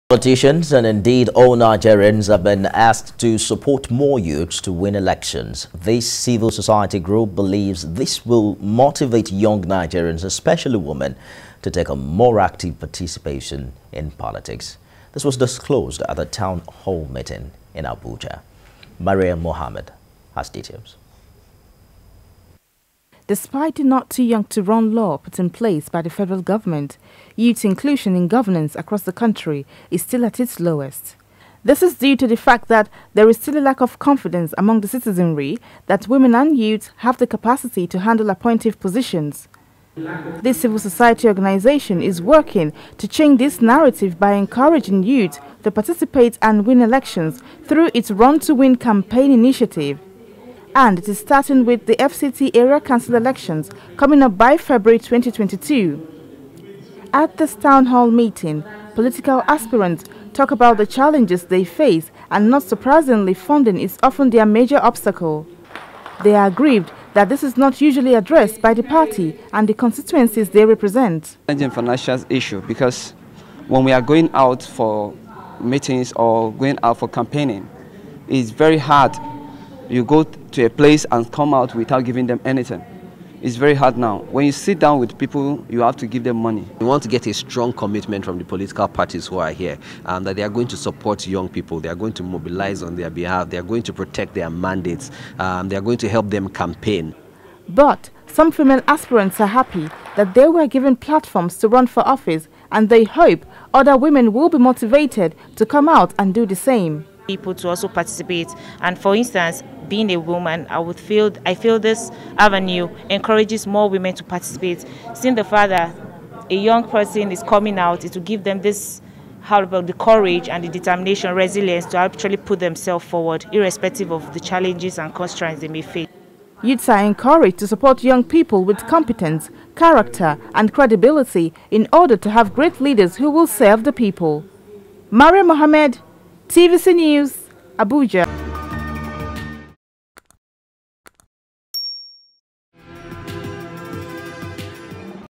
CSO Urges Citizens To Support Young Aspirants To Win Elections
Politicians and indeed all Nigerians, have been asked to support more youths to win elections.
This Civil Society Group believes this will motivate young Nigerians especially women to take a more active participate in politics.